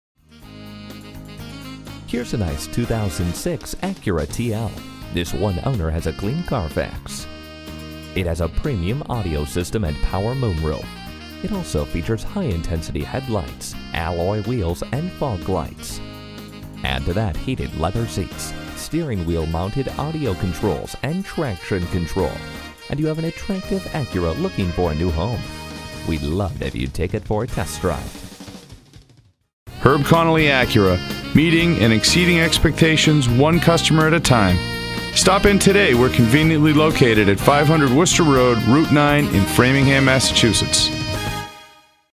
2006 Acura TL Framingham Natick Marlborough MA, MA A020653A - SOLD
Year: 2006
Make: Acura
Model: TL
Trim: Base
Engine: 3.2L V6 SOHC VTEC 24V
Transmission: 5-Speed Automatic
Color: Gray
Mileage: 77789
Stock #: A020653A
One Owner - Clean Carfax - Power Moonroof - HID Headlights - Acura/ELS Prem AM/FM w/XM Satellite Radio - Alloy wheels - Front fog lights - Heated front seats - Perforated Leather Seat Trim - Steering wheel mounted audio controls. 21 service records here at our dealership - cleanest 2006 TL you have ever seen! No gimmick - total cream puff! Take a look at this fantastic 2006 Acura TL. Roll down the street in the lap of luxury. J.D. Power and Associates gave the 2006 TL 4.5 out of 5 Power Circles for Overall Performance and Design. Remember at Herb Connolly Auto Group "WE MAKE THE DEALS OTHER DEALERS DON'T".

Address:
500 Worcester Road Route 9 E